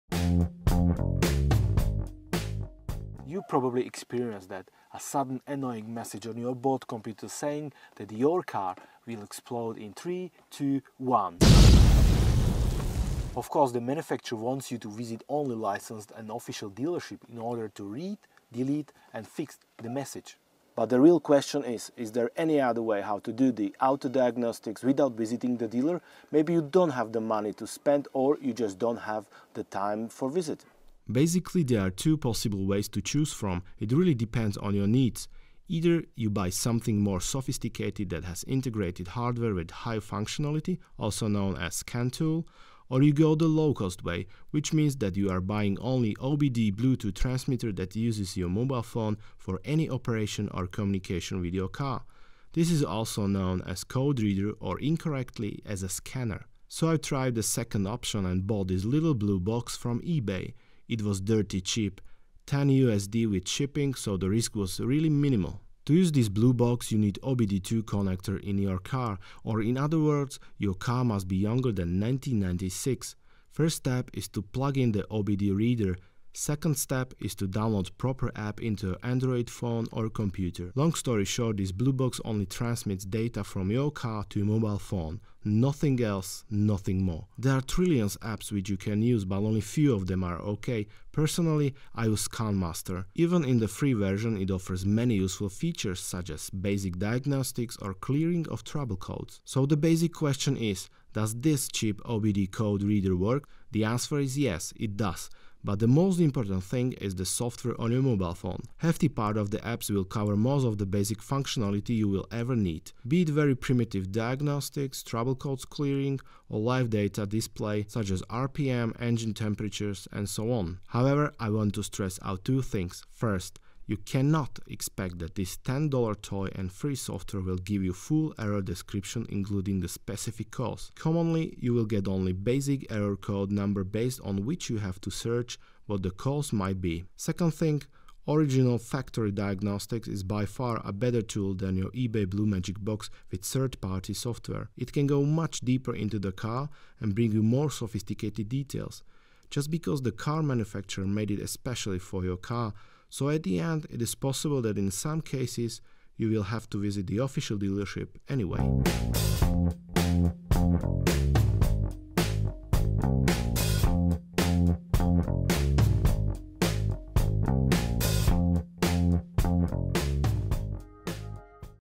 Ebay OBDII bluetooth code readers. Do they work?
Can 10$ OBDII car diagnostics device from eBay replace more sophisticated and much more expensive tools? There is only one way how to find out...